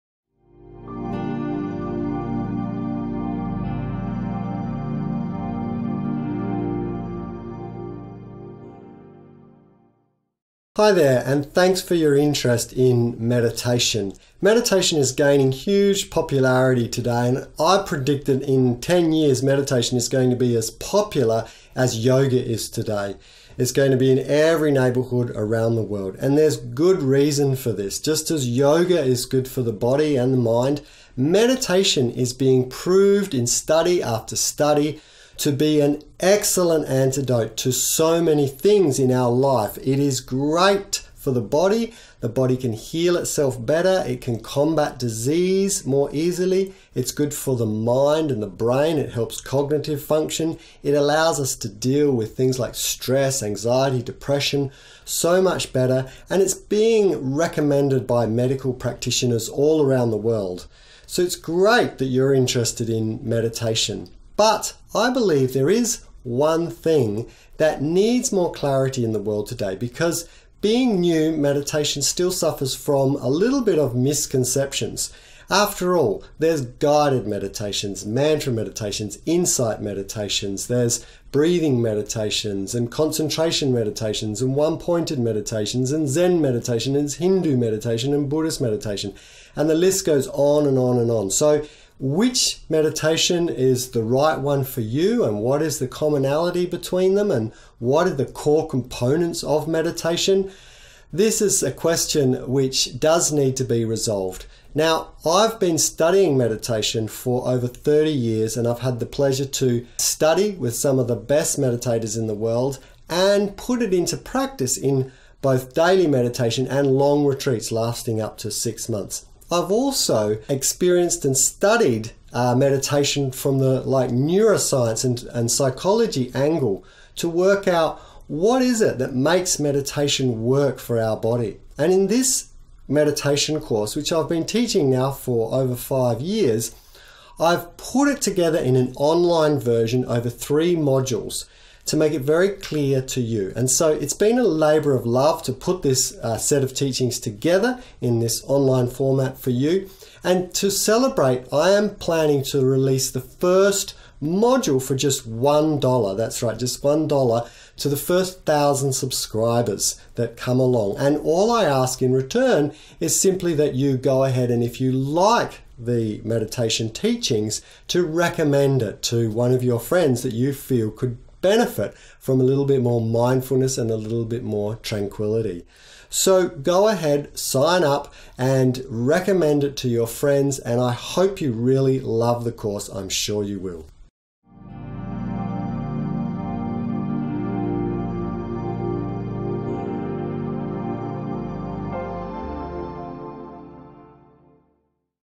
Meditation course intro
This is the sales video for my new online mediation course